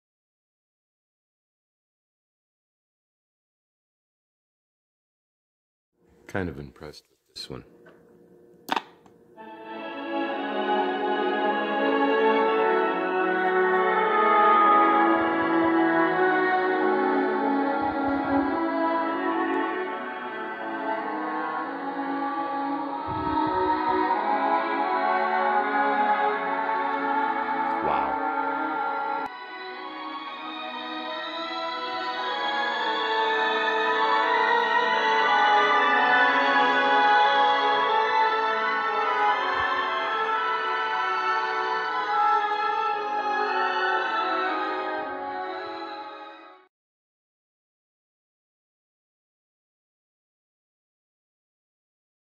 MULTITECH Skywatch Pro Vintage Transistor Radio
Another rare vintage pocket transistor radio. The Skywatch Pro has Vhf Air Band, FM, LF and Weather Bands. This is a well built radio from Japan. It has amazing audio. Comment if you own one or know more about this unique radio.